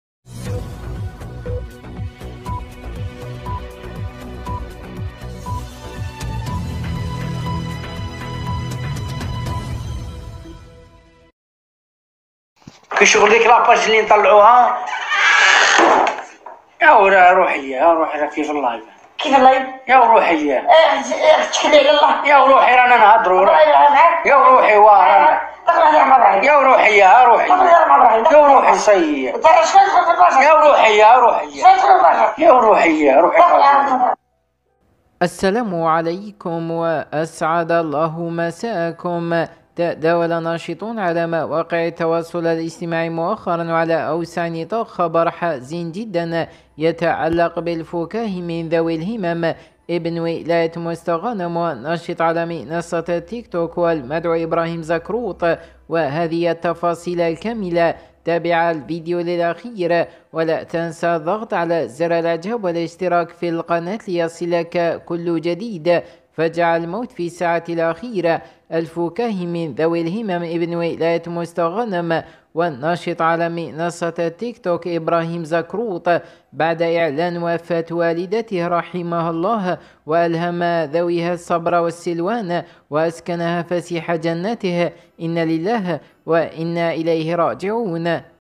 عاجل جدا 🔴 المـ.وت يفجع الفكاهي و التيكتوكر المعروف ابراهيم زكروط...انا لله وانا اليه راجعون
وفاة والدة الفكاهي الجزائري من ذوي الهمم ابراهيم زكروط ابن بوسكي ولاية مستغانم
الفقيدة فرعون زوجة محمد زعروط تم تشييع جنازتها